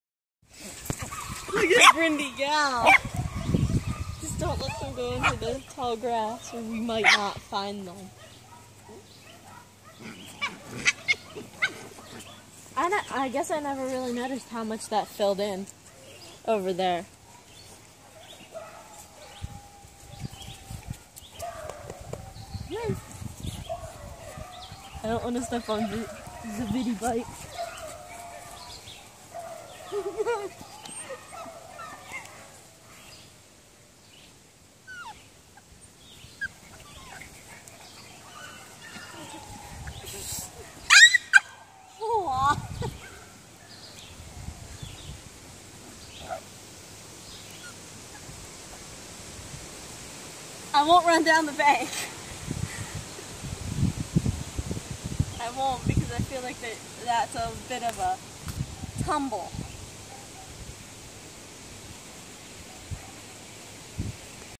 Brindy and Cami playing. 12 week old foster puppies
12-week old mixed breed foster puppies.  They're ready for their new homes. 5-29-17